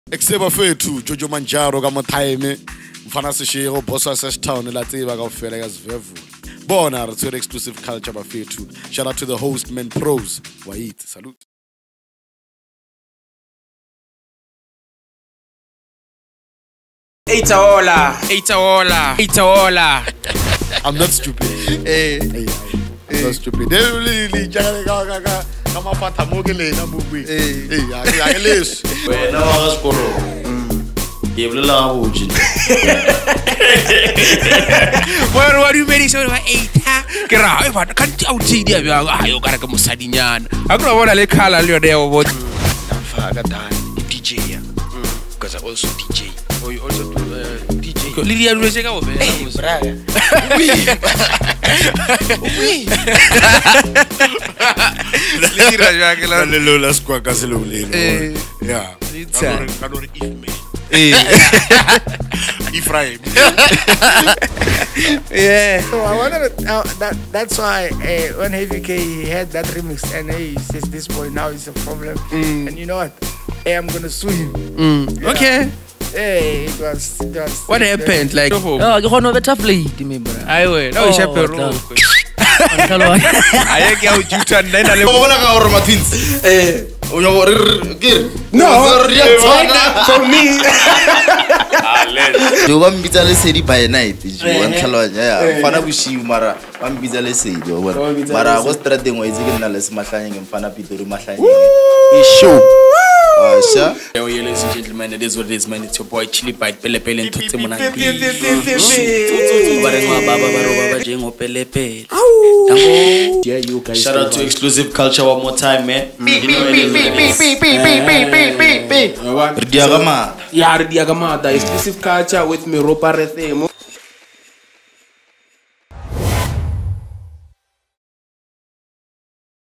Chilli Bite and Less Mahlanyeng|Jojo Manjaro|DJ Ace|MJ Lort|Majestigg|Thulasizwe Episode coming soon
We are excited to announce the Podcast is celebrating one year since we started with this journey of podcasting.

This new season features some of the well-known celebrities.